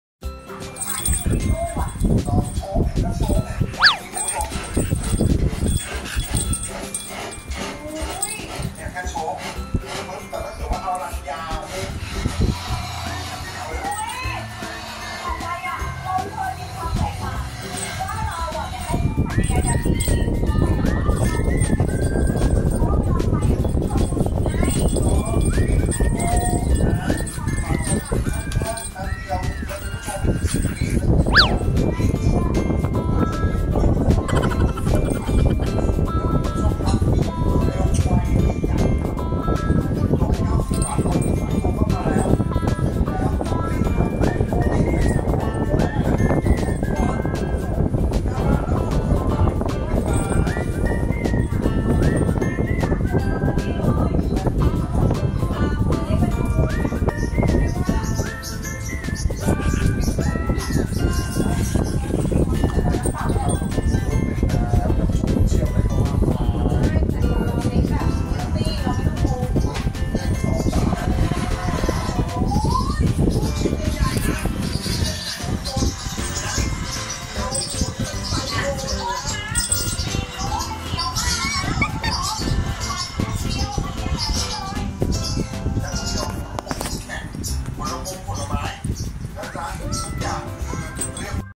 She Wanted To Play Too
NhingNhing was watching Daddy and Billion playing Tug and she couldn't resist to play with us! 🤣😁